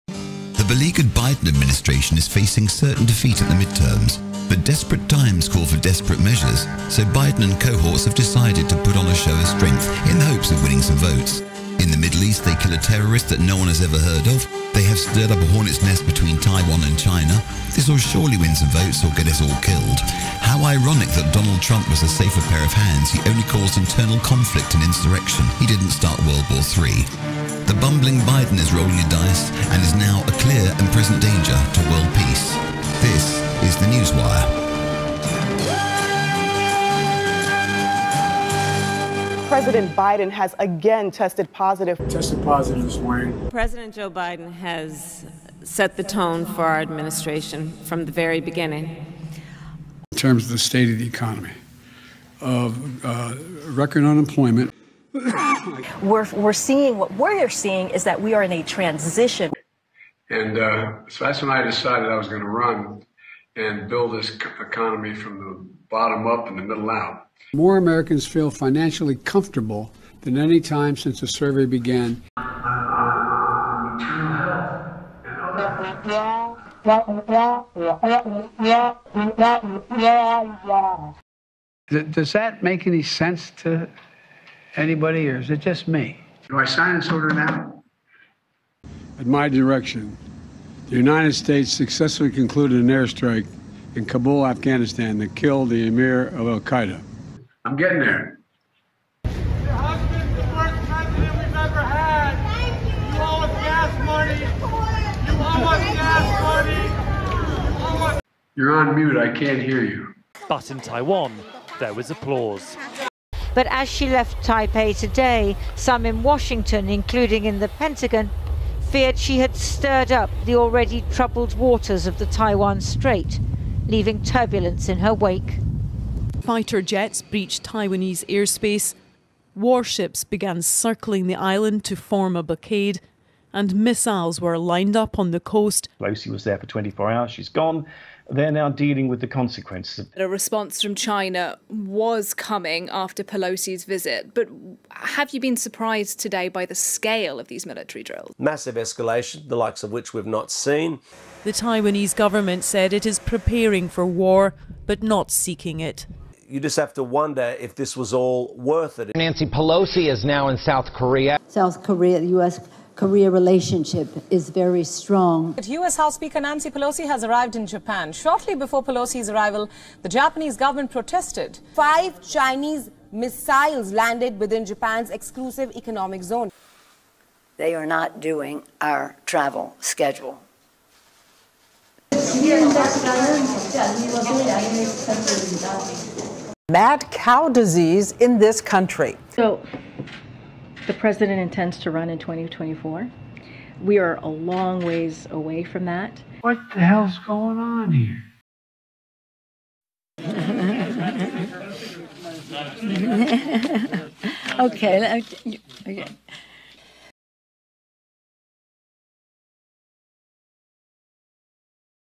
Aftermath of Pelosi visit. Biden threat to world peace. China missile strikes. [Crazy Nancy]
The beleaguered  Biden administration is facing certain defeat at the midterms.

Desperate measures, Biden and cohorts have decided to put on a show of strength in the hopes of winning some votes.

In the middle east, they kill a terrorist that no one had ever heard of.  They increase tensions between Taiwan and China.

Ironic that Donald Trump was a safer pair of hands.  He only caused internal conflict and insurrections.  He did not start World War 3.

The bumbling Biden is rolling the dice and is now a clear and present danger to World peace.

Newswire editorial.